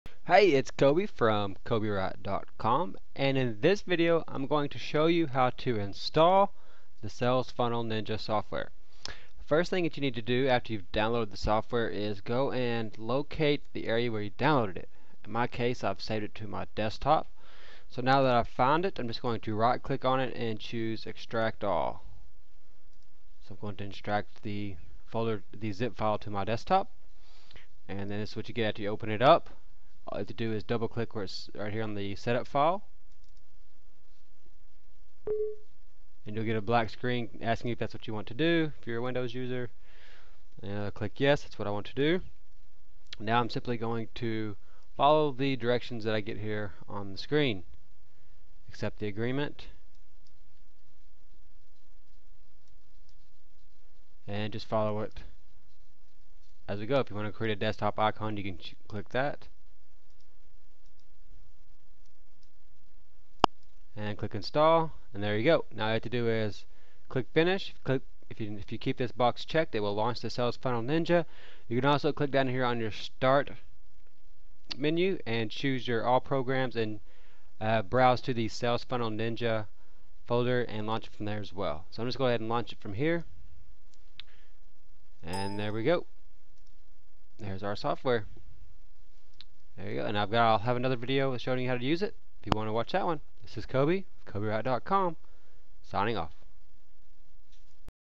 How To Install Sales Funnel Ninja Software
Sales funnel ninja software by Coby Wright will help you make money online with affiliate marketing by building a squeeze page and meta redirect with countdown that you can use to promote affiliate products that have a sales video or to promote or give away anything that uses a video.

Coby Wright
review
software
sales funnel ninja
list building
video marketing
make money online
product creation
marketing
business
money